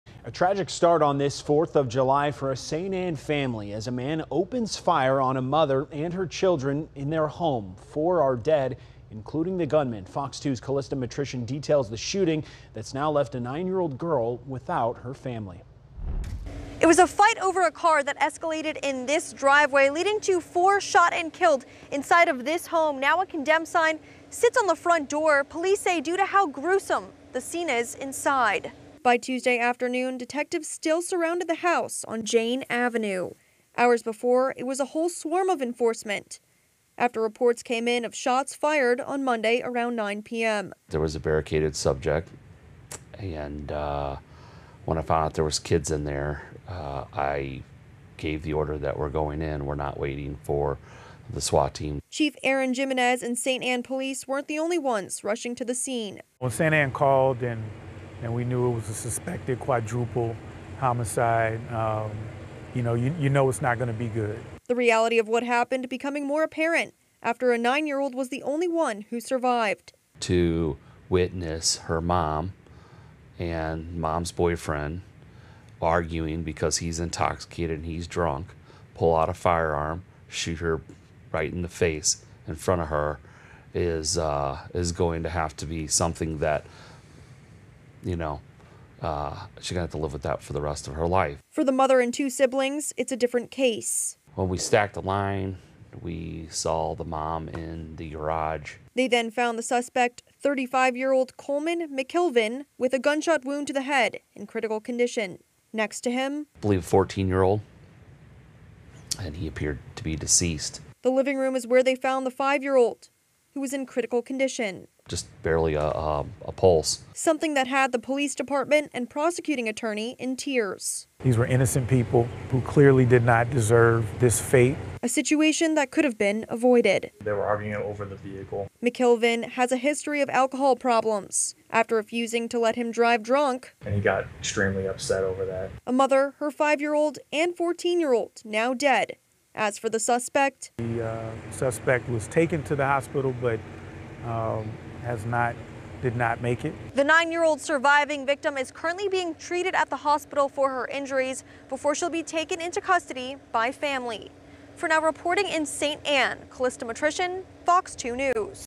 Woman and children killed, teen injured in St. Ann shooting
A woman and two of her children were shot dead Monday evening in St. Ann, and a fourth shooting victim hospitalized.